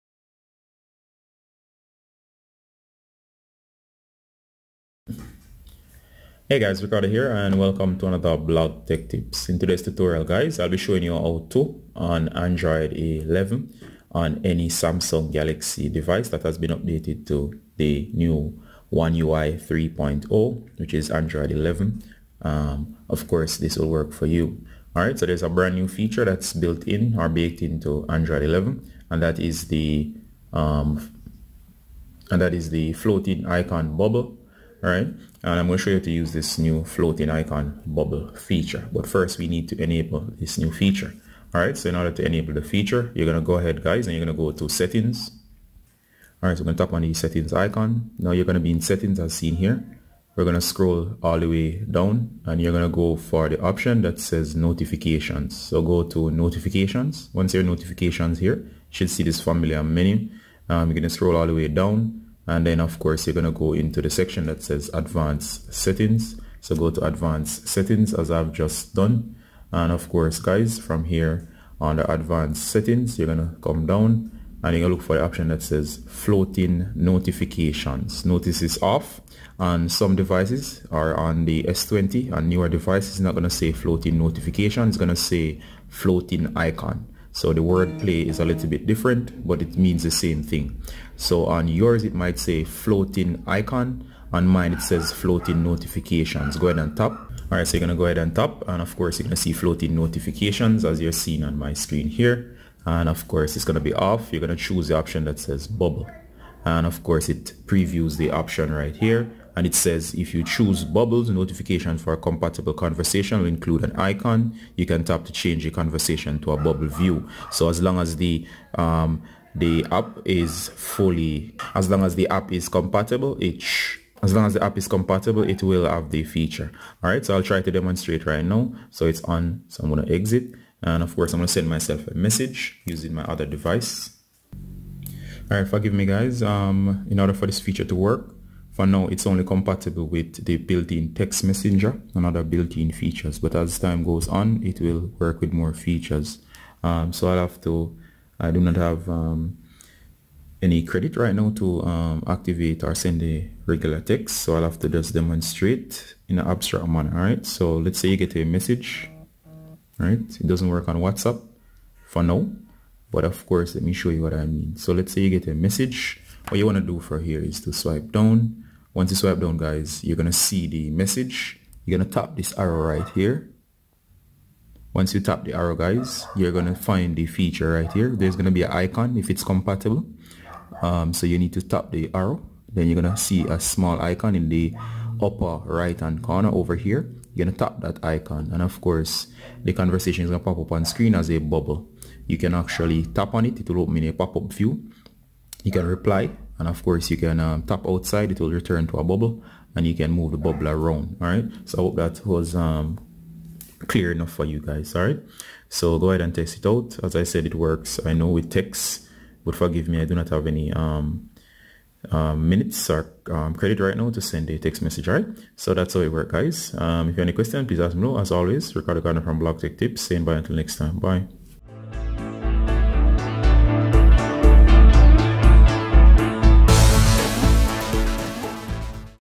Floating Icon Bubbles Android 11 {New Feature}
Cool Floating Icon Bubbles Android 11 new feature allows you to keep messages on screen as a Bubble and have a quick reply window reply and continuing the conversation on your Samsung Galaxy. This feature is also known as Floating Notifications on some devices.